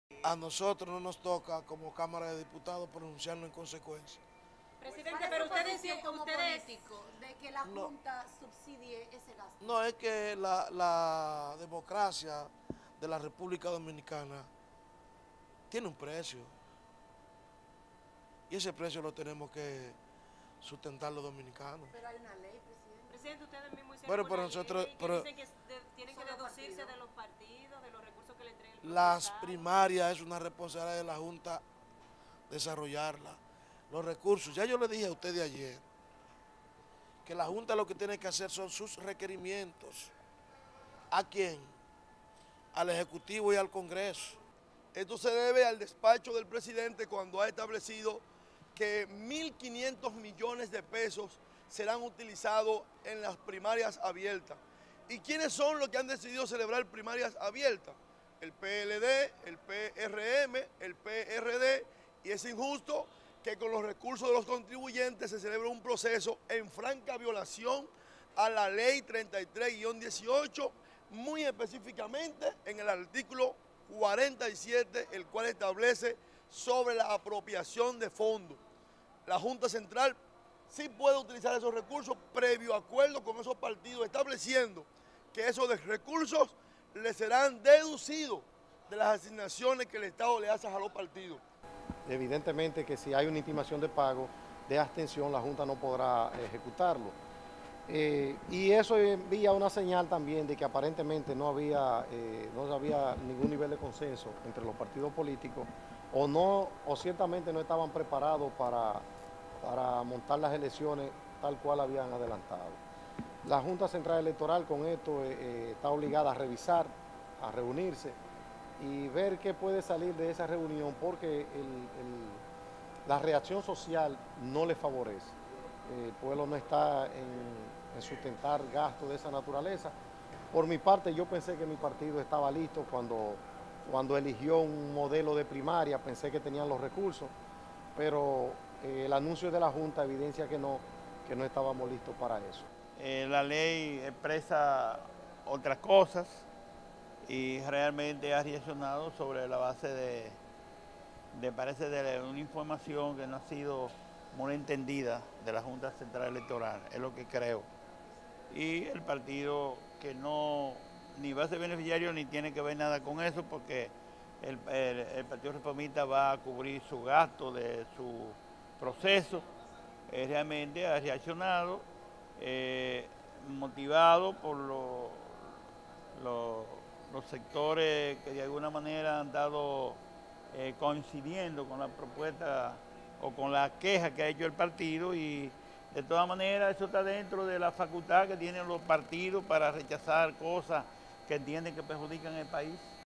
Reacciones: JCE no debe usar fondos en Primarias
Diversas reacciones ha generado en los congresistas la intimacion de Partido Reformista Social Cristiano a la Junta Central Electoral para que se abstenga de usar fondos públicos en la celebración de las primarias de los partidos políticos.

Otros opositores critican que la intimacion retrasa el proceso y revela que no estaban preparados para la aprobación, mientras, el presidente de la Cámara Baja, Radhames Camacho, invitó a la Junta a presentar sus requerimientos financieros al Poder Ejecutivo y al Congreso Nacional y recordó que la democracia tiene un costo que debe ser asumido.